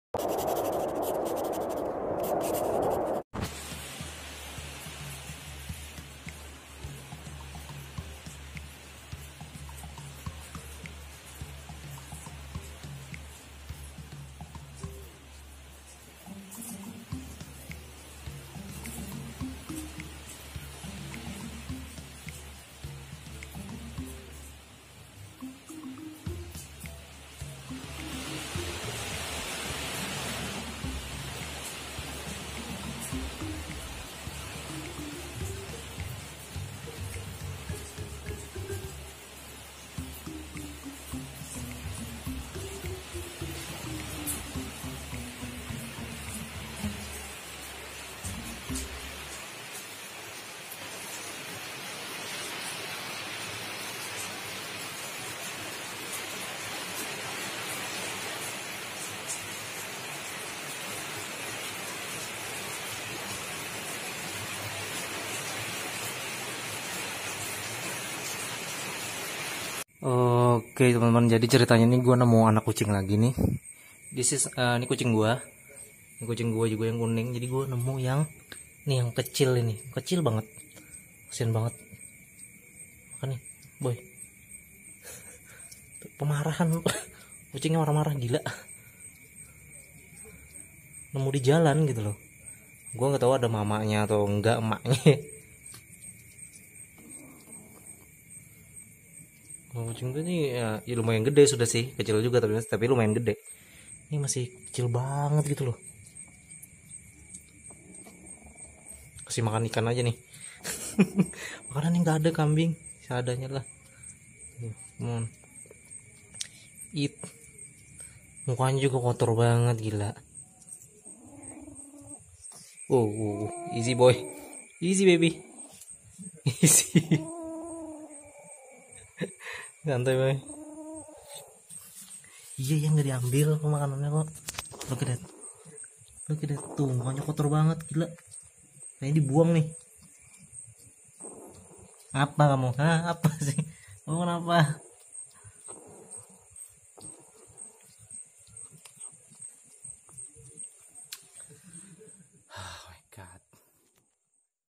Tau Kucing Gak Youtube Tolol 😱😱
OMG jadi cerita nya hari waktu ujan deres gw mergokin mereka bgituan..
dsar bocil lucknut ☺️🤣😂😂

April Foollssss 😂😂